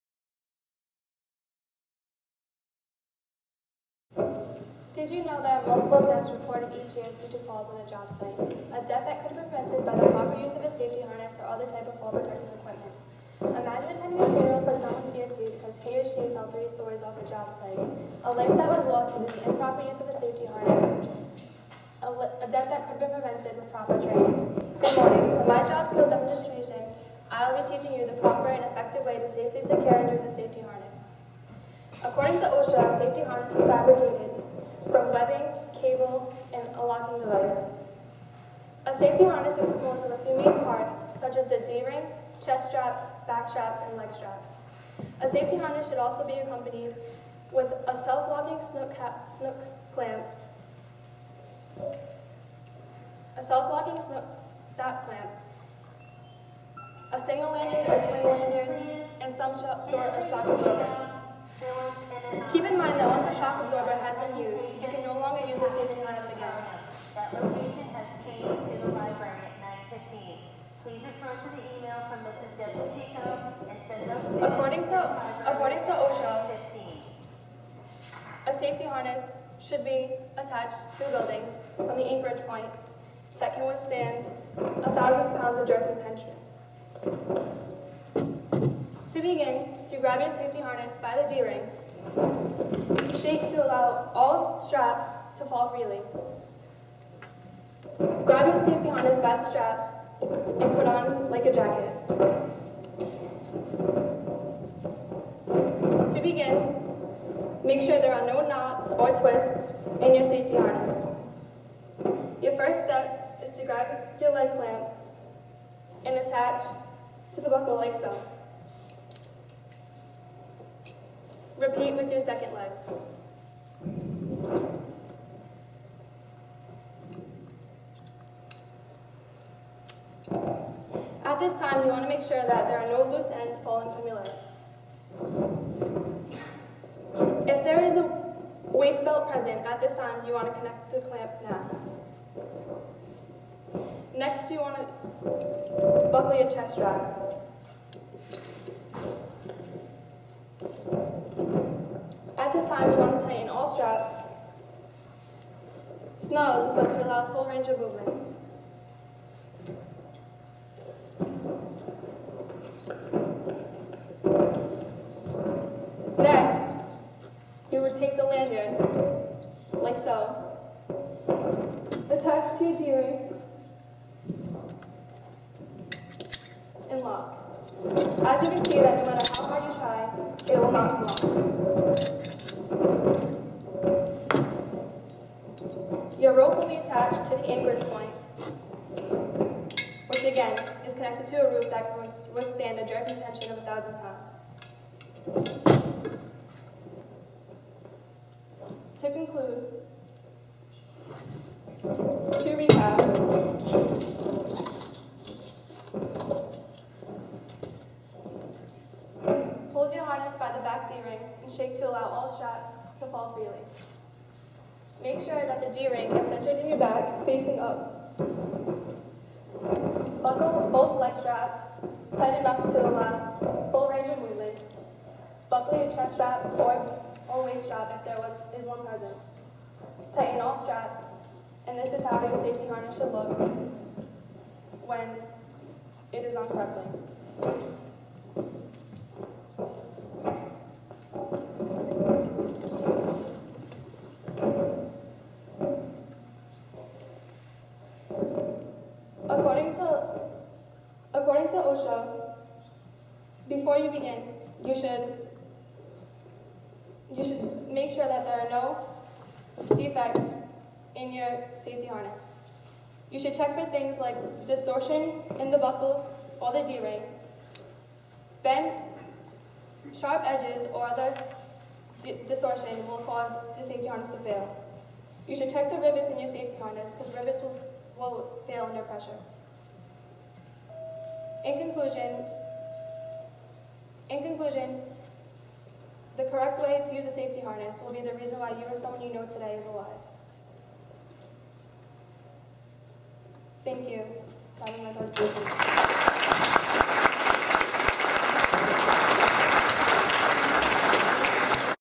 Diman SkillsUSA Job Skills Demo - Nicole Amaral (2014)
Video shot by members if Diman's Audio Visual Club